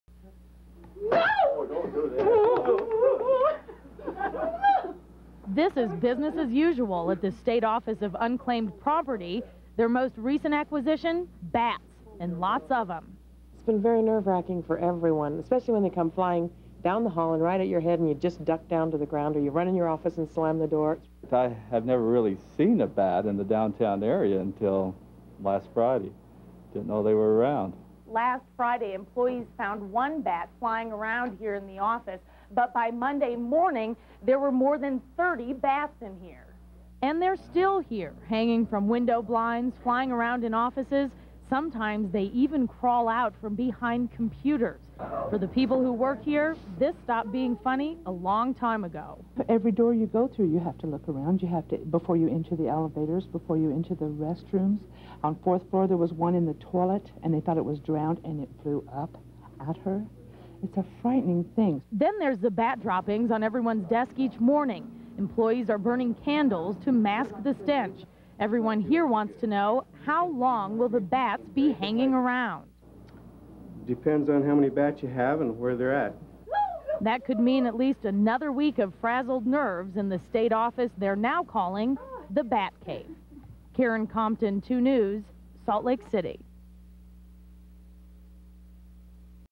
Bats Invade Office!
Amusing feature story about bats that took over an office building in downtown Salt Lake City.  The bats certainly kept things interesting with workers ducking and dodging the low flyers.  (1997)